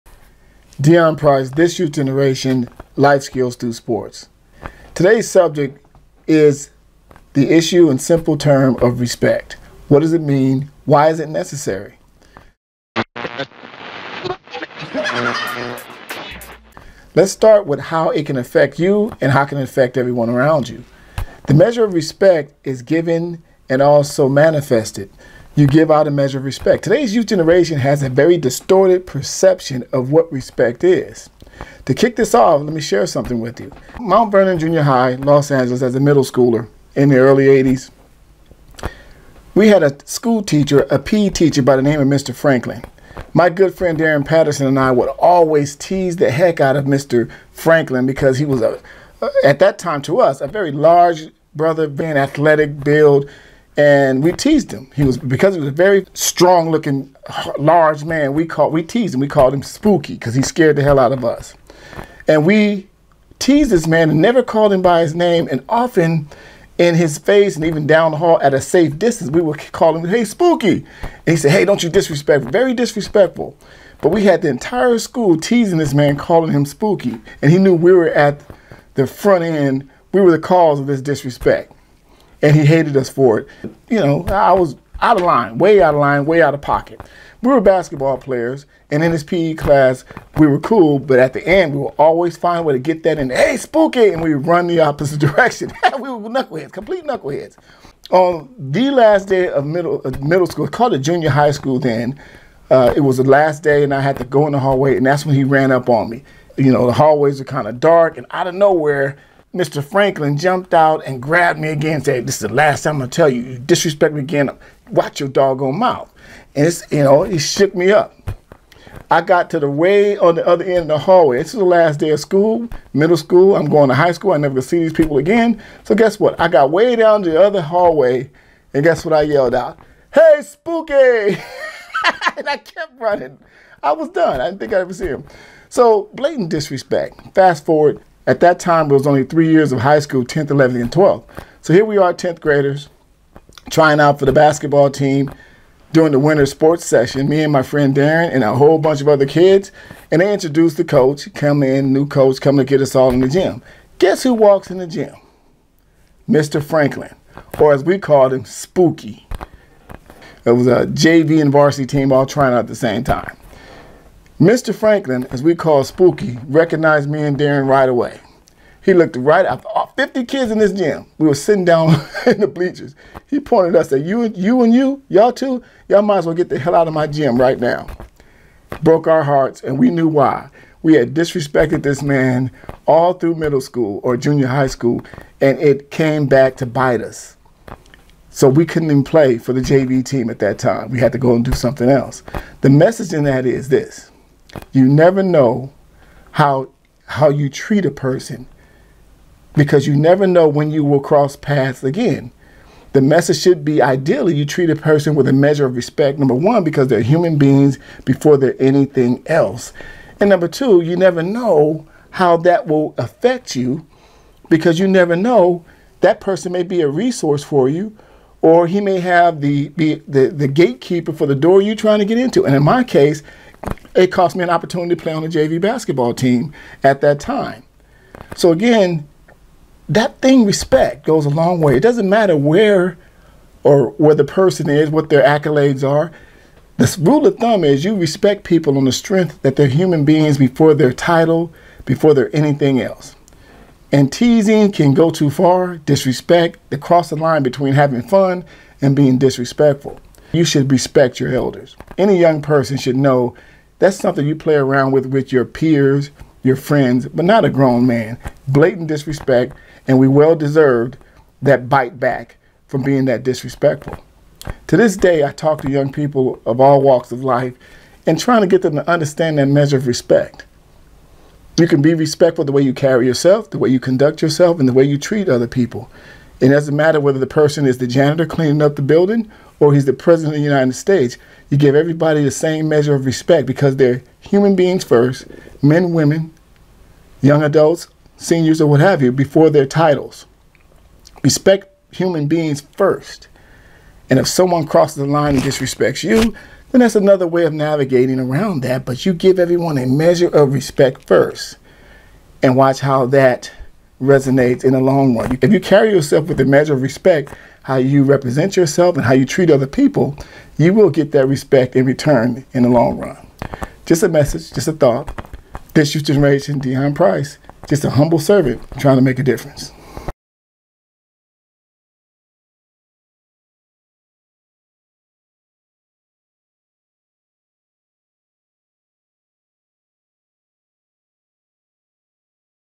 tyg life skills through sports respect
Author & Youth Life Skills Coach Deon D. Price, delivers another episode of Life skills through Sports that focuses on the value of RESPECT.